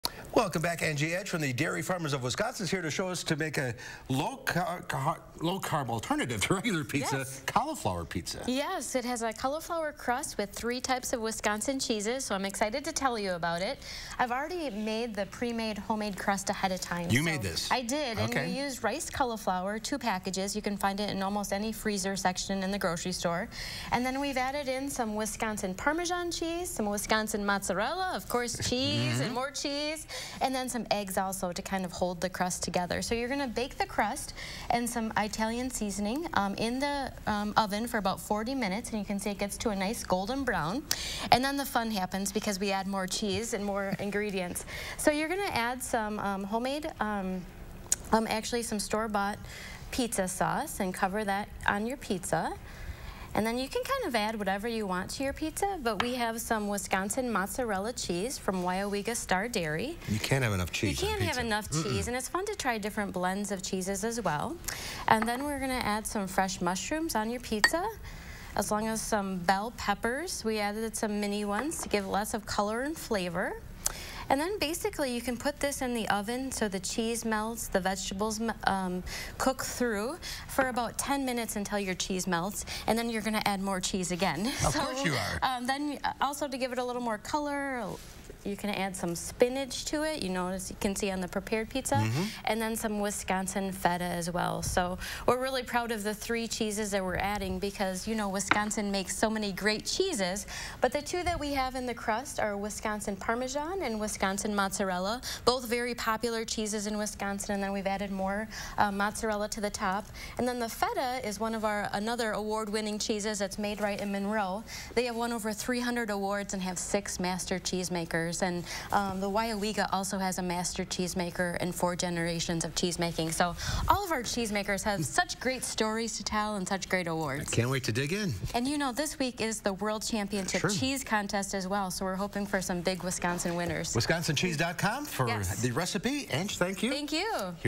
How to make cheesy cauliflower pizza
Angie Edge from the Dairy Farmers of Wisconsin shows us a low-carb alternative to pizza.